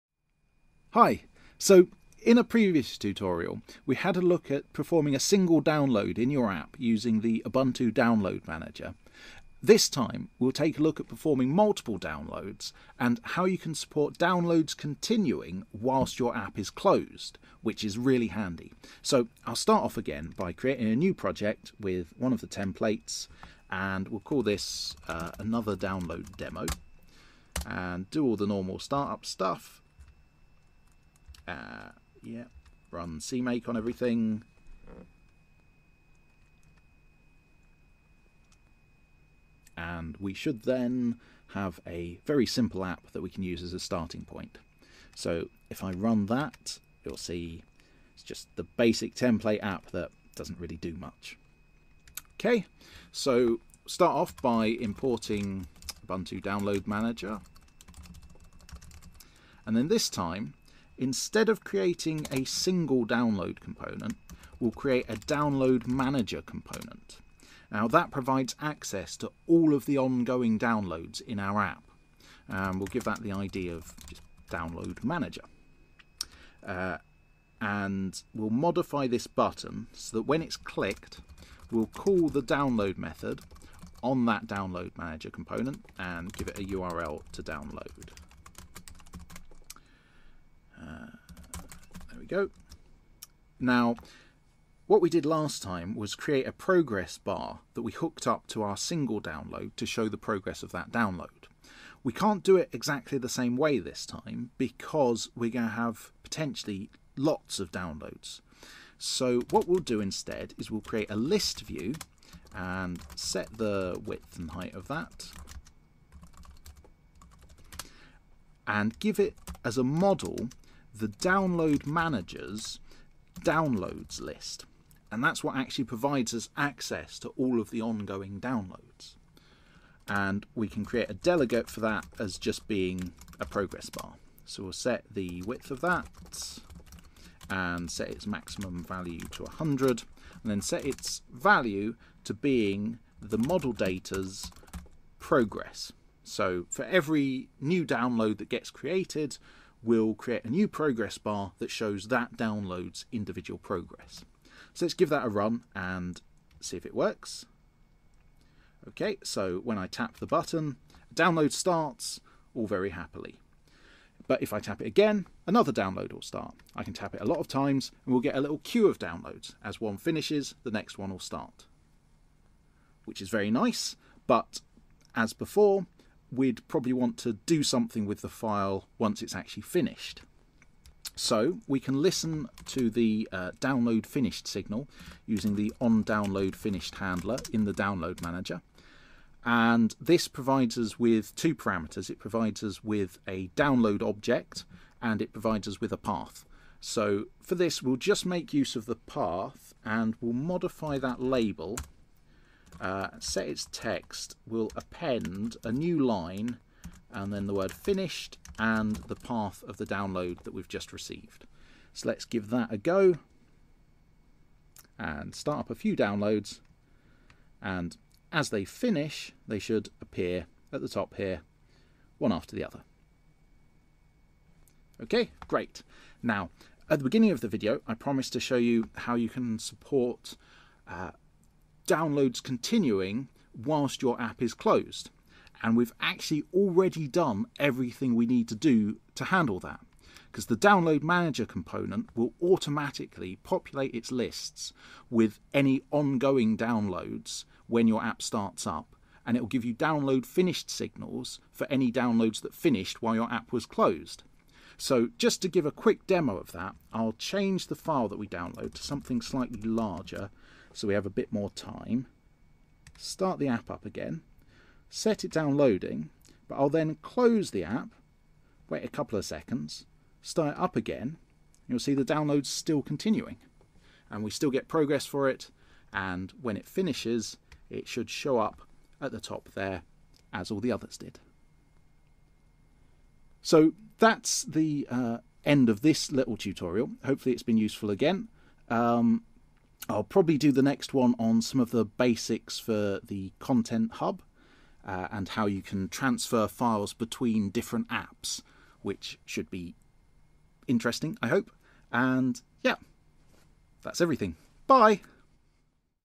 Ubuntu App Development Tutorials: Handling Multiple Downloads & Downloading While Your App Is Closed
Another short tutorial covering how to handle multiple downloads within your apps using the Ubuntu download manager and how your app can support downloads continuing whilst it's closed.

The first video in this series showing the basics of using the download manager and how to integrate it with the transfer indicator can be seen